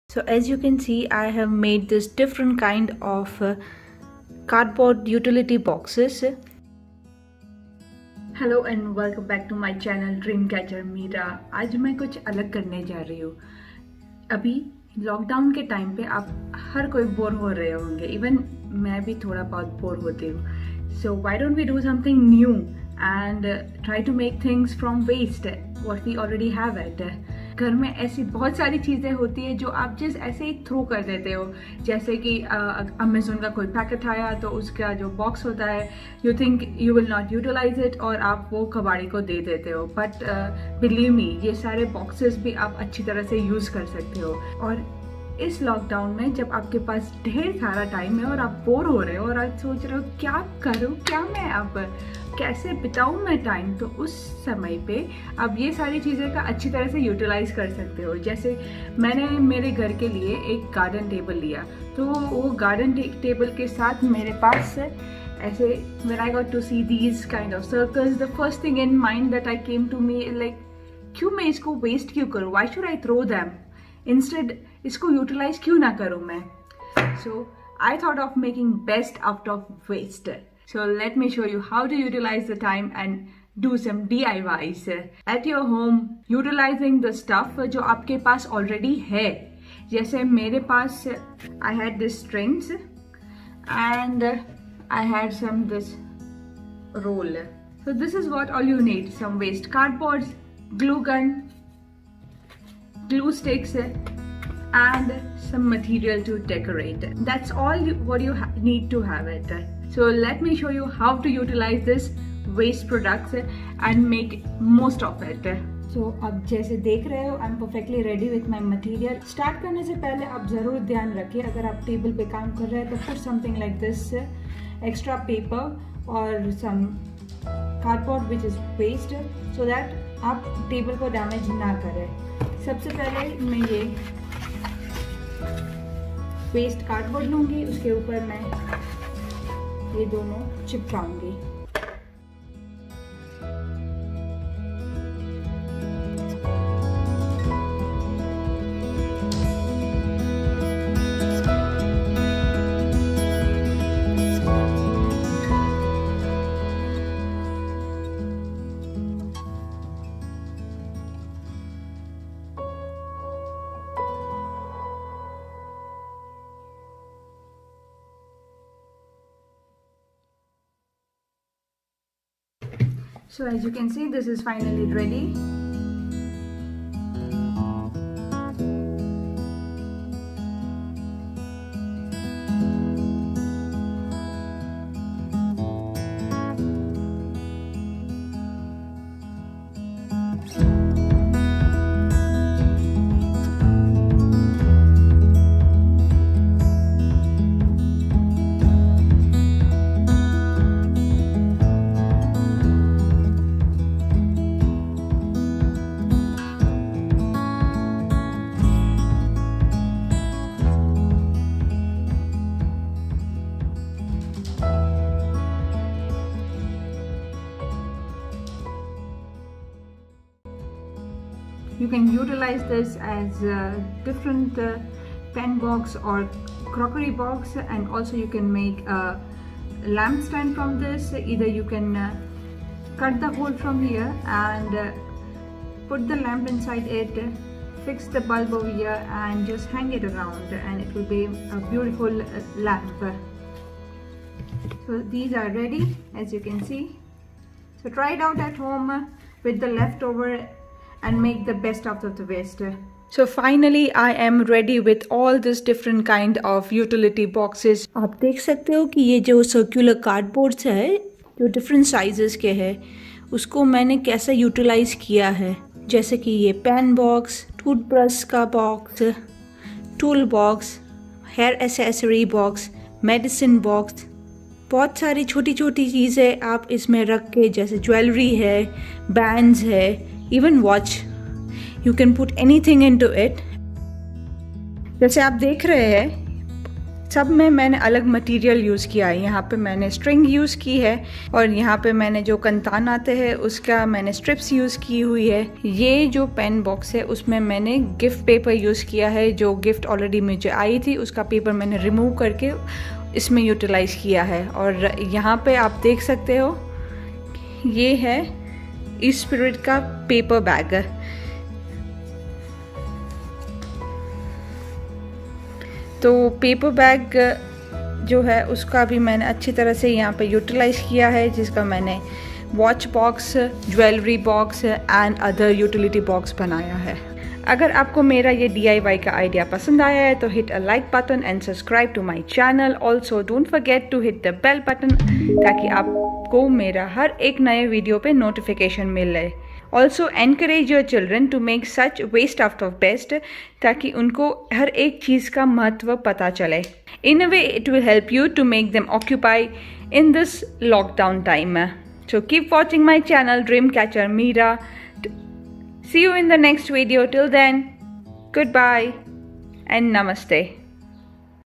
Easy DIY Organizer Box || DIY Storage BOX || Best out of Waste || Lockdown Activity #3
Here is how you can Utilize your and your Children's Time in this Lockdown by making this easy DIY Organizer or Storage Box and at the same time make the Best out of Waste rather than throwing it away. Make your Kids Creative and decorate their Rooms with this beautiful easy DIY Organizers.

 Air BnB : get 34 Euro on signin up and by First Booking
Click here for more exciting Videos-